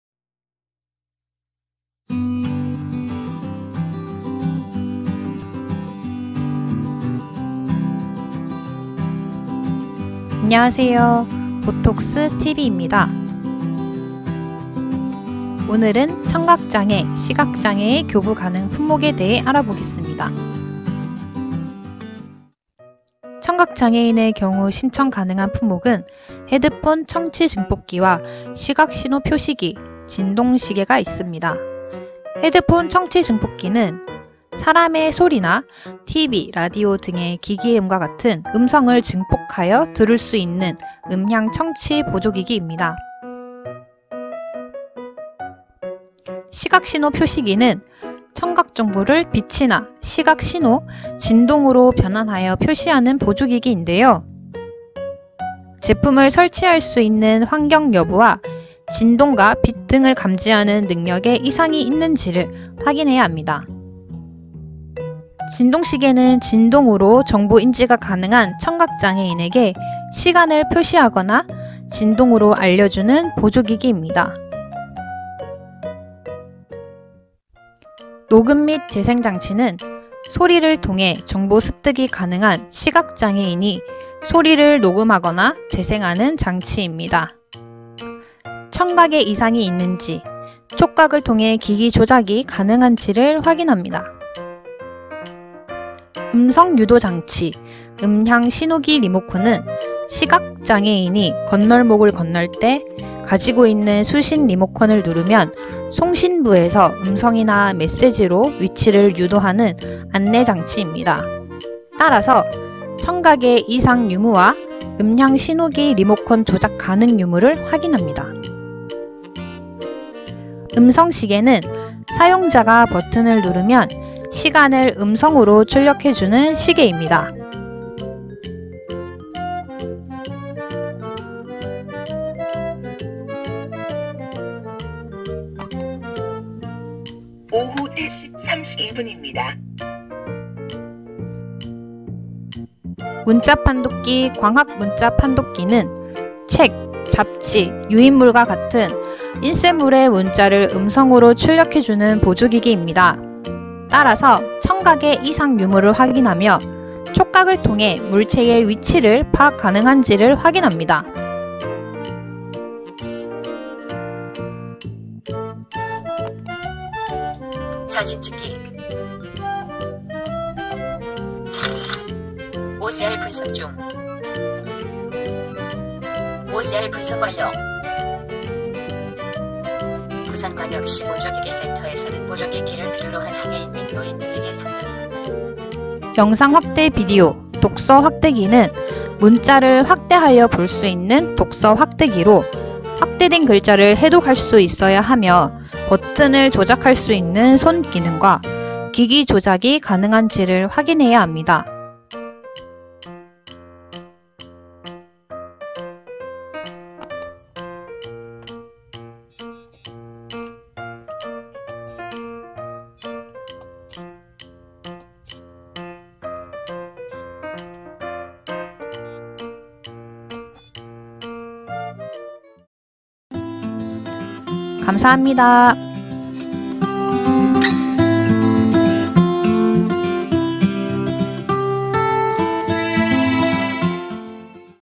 [보톡스] 2020 장애인보조기기 교부사업 장애유형별 품목안내(청각, 시각) 4편
[보톡스]
- 영상에 사용된 사진은 국립재활원 2020년 보조기기 교부사업 품목안내서에 등록된 제품사진, 장애인복지 사업안내에 게시된 사진 및 부산광역시 보조기기센터에서 보유하고 있는 보조기기를 사용하였습니다.

보.톡.스!!
보조기기 토크 & 스토리~

부산광역시보조기기센터의 전문가들이 모여
보조기기에 담긴 우리의 찐!! 스토리를 들려드립니다.